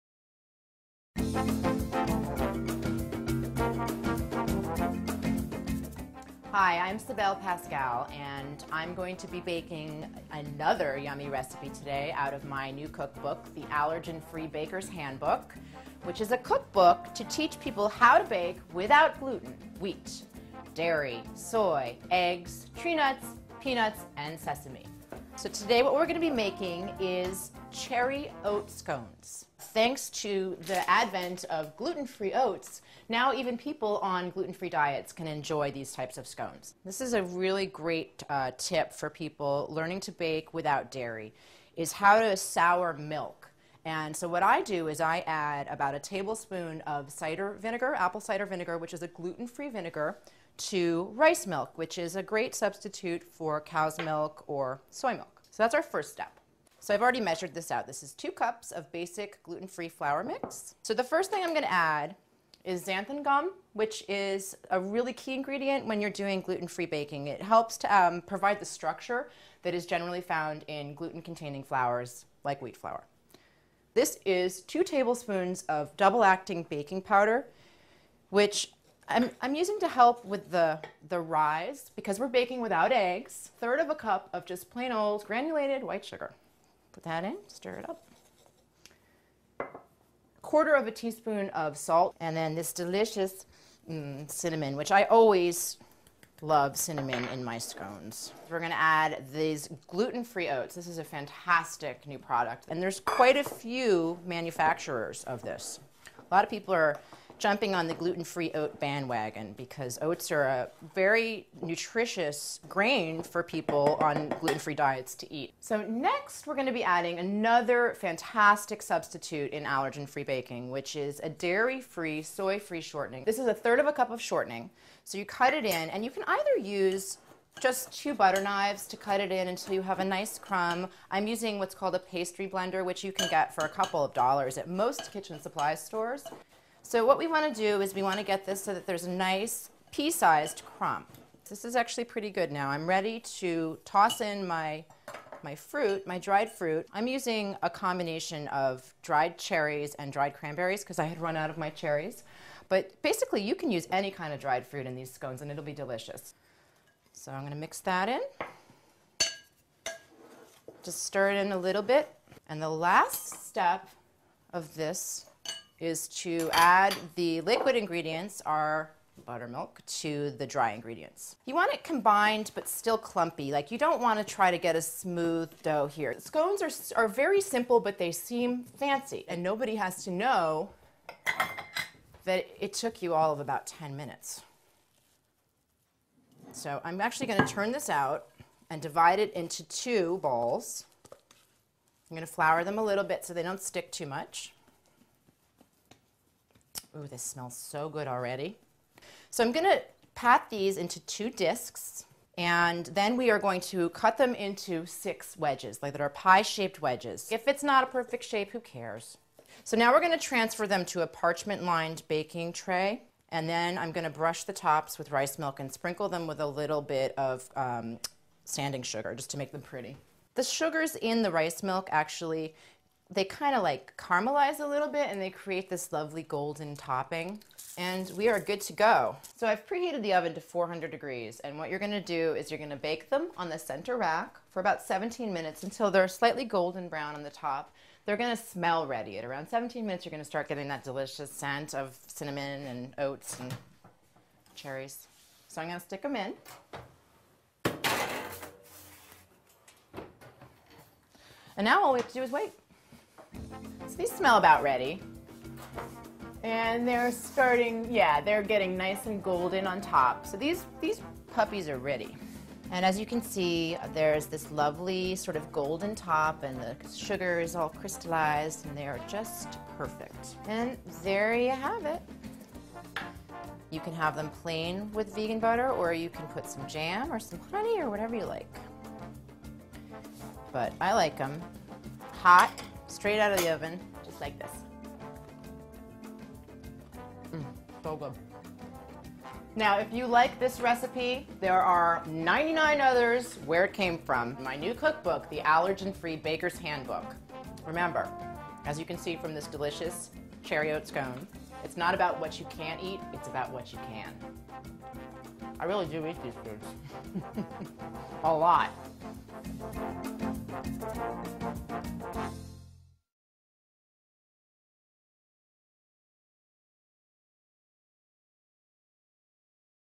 Cybele Pascal makes Gluten-Free, Vegan Cherry Oat Scones
Award-winning allergen-free cookbook author, Cybele Pascal makes gluten-free, vegan, allergen-free Cherry Oat Scones.  Allergen-Free baking, gluten-free baking, vegan baking, allergy-friendly baking, allergy friendly